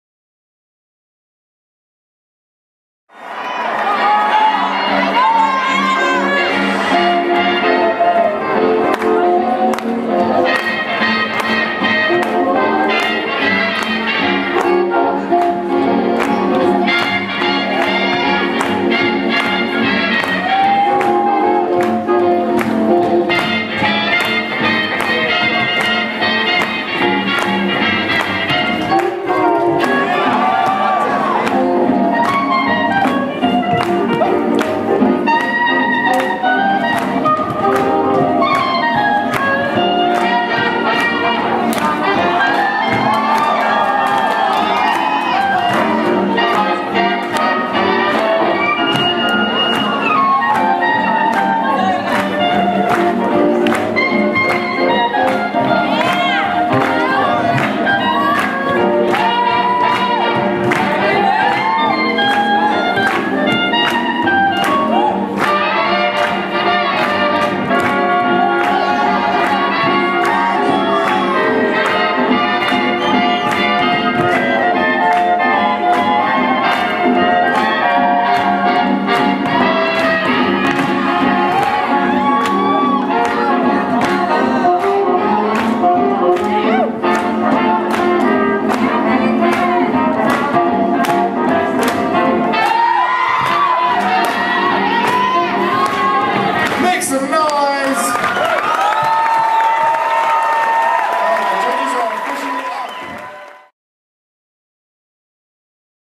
2011 ILHC Champions Strictly Lindy Prelim - Heat 1 Song 2
Heat 1 Song 2 of the Champions Strictly Lindy Prelim at the International Lindy Hop Championships (ILHC) on Saturday, August 27, 2011 in Alexandria, VA.